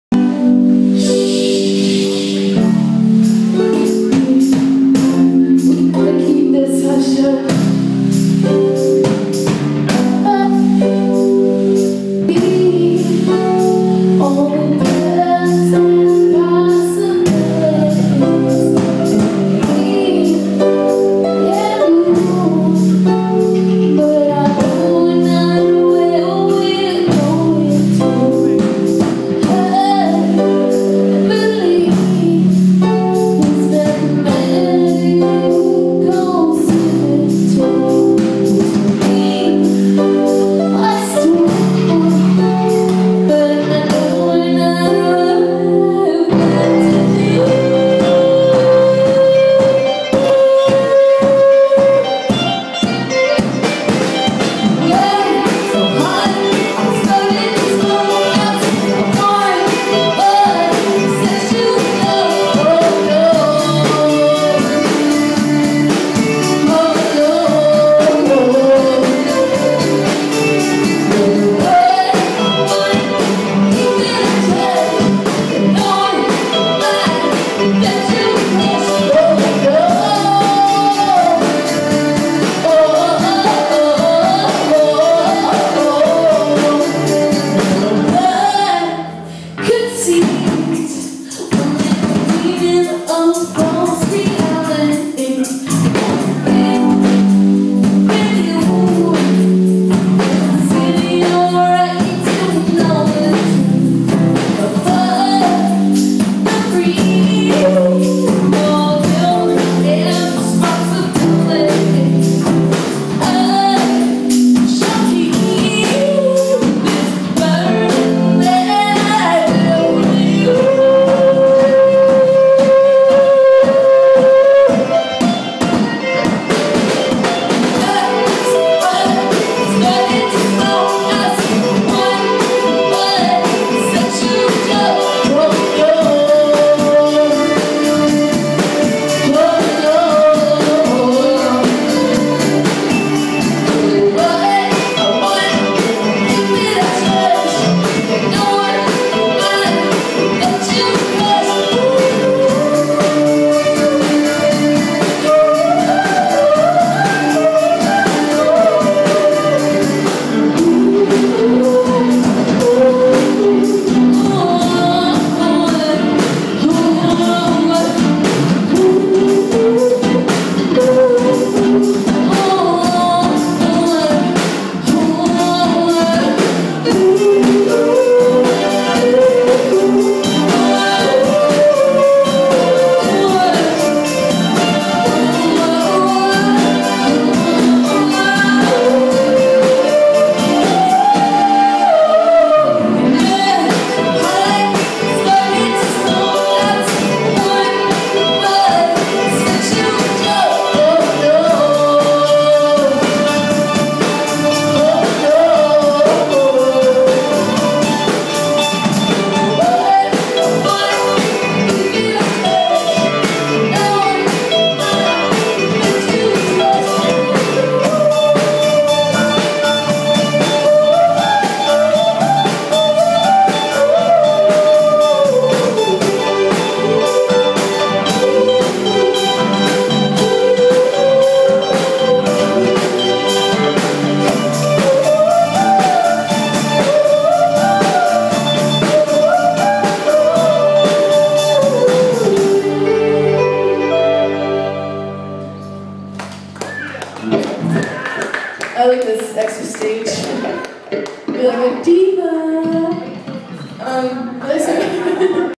Friends - Live at Sinister Foxy Stage/Synth by Southwest
Friends played the Sinister Foxy Stage at Synth by Southwest on Wednesday, March 16, 2011.